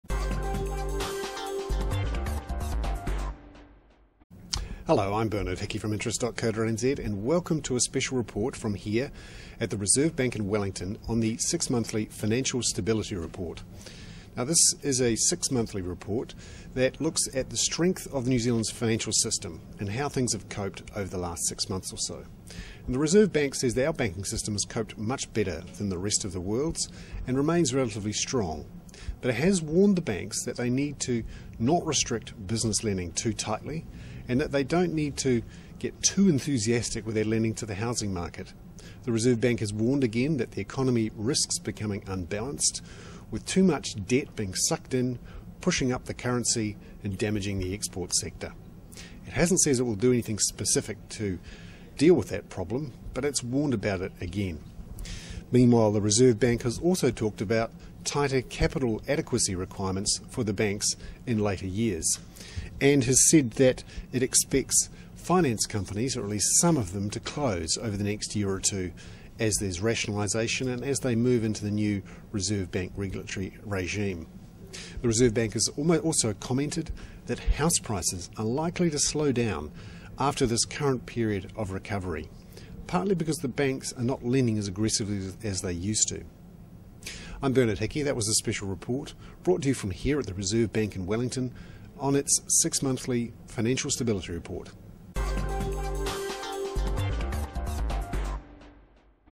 November 11, 2009: RBNZ Financial Stability Report
Bernard Hickey is at the Reserve Bank of New Zealand for its six-monthly Financial Stability Report